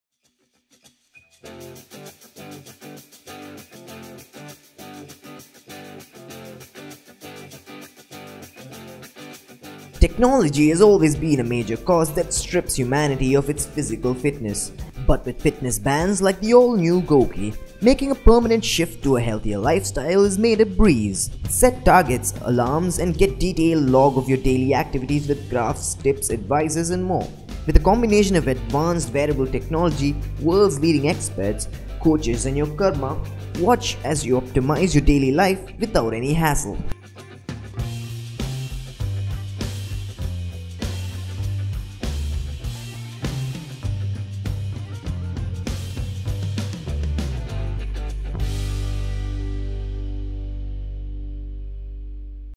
GOQii fitness smartband
Technology has always been a major cause that strips humanity off its physical fitness. But with a fitness bands like the all new GOQii, making a permanent shift to a healthier lifestyle is made a breeze.